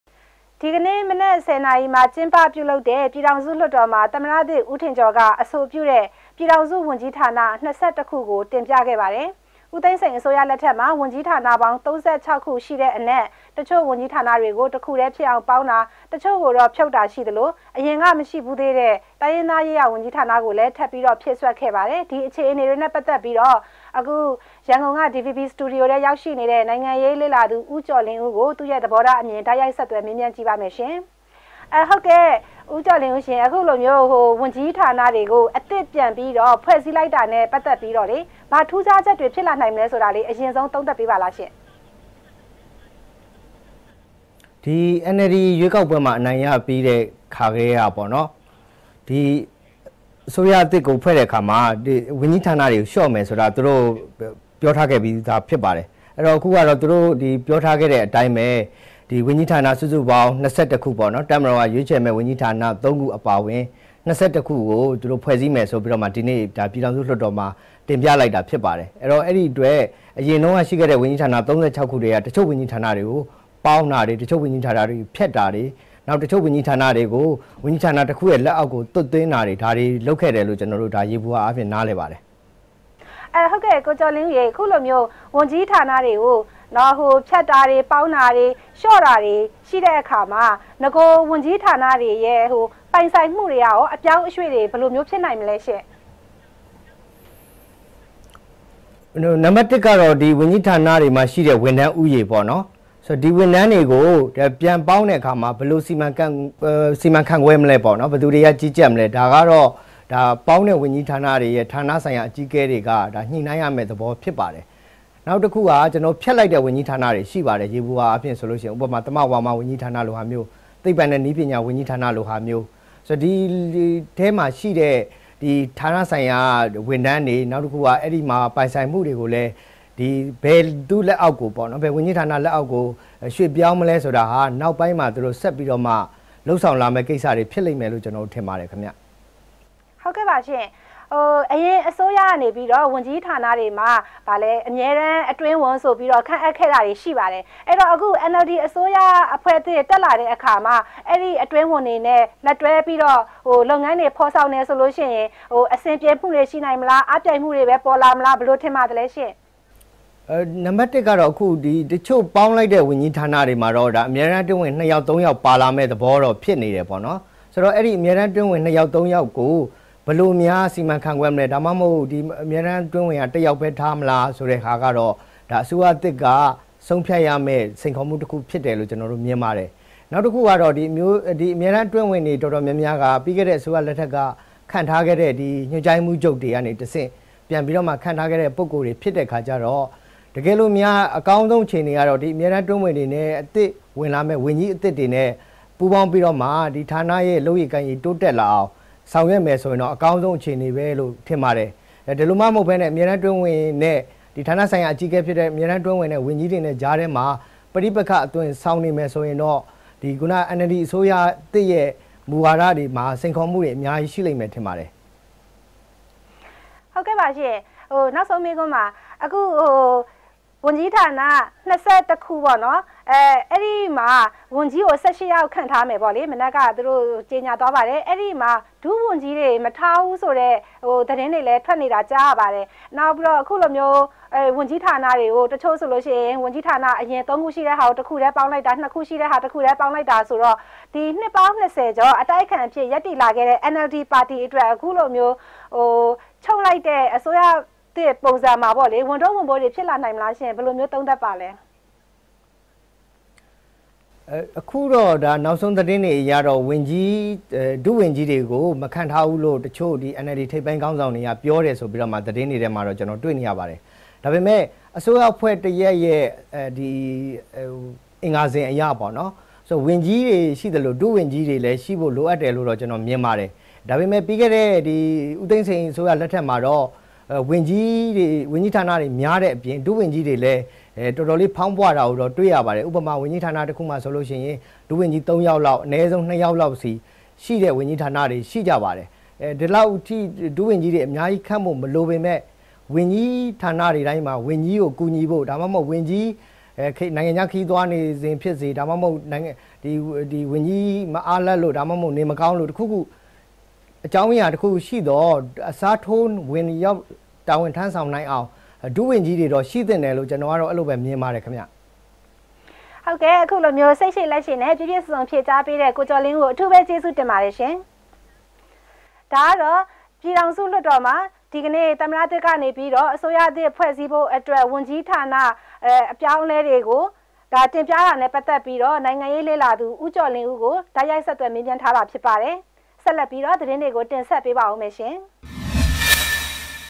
DVB Live - အစိုးရသစ္ ၀န္ၾကီးဌာန အေျပာင္းအလဲေတြအေပၚ သုံးသပ္ခ်က္
ဒီကေန႔ မနက္ ၁၀ နာရီမွာ က်င္းပျပဳလုပ္တဲ့ ျပည္ေထာင္စုလႊတ္ေတာ္မွာ သမၼတသစ္ ဦးထင္ေက်ာ္က အဆိုျပဳတဲ့ ျပည္ေတာင္စု၀န္ႀကီးဌာန ၂၁ ခုကုိ တင္ျပပါတယ္။ ။ ဦးသိန္းစိန္ အစိုးရလက္ထက္မွာ ၀န္ႀကီးဌာနေပါင္း ၃၆ ခုရွိတဲ့အနက္ တခ်ဳိ႔၀န္ႀကီးဌာနေတြကို တခုတည္း အျဖစ္ေပါင္းတာ၊ တခ်ဳိ႔ကို ျဖဳတ္တာရွိသလို အရင္က မရွိဖူးတဲ့ တိုင္းရငး္သားေရးရာ ၀န္ႀကီးဌာန ကို ထပ္မံျဖည့္စြက္ခဲ့ပါတယ္။ ဒီအေျခအေနေတြ နဲ ့ပက္သက္ပီး ရန္ကုုန္စတူဒီယိုုထဲကိုုေရာက္ေနတဲ့ ႏိုုင္ငံေရးေလ့သူ ဦးေက်ာ္လင္းဦးကိုု တိုုက္ရိုုက္ဆက္သြယ္ေမးျမန္းထားပါတယ္။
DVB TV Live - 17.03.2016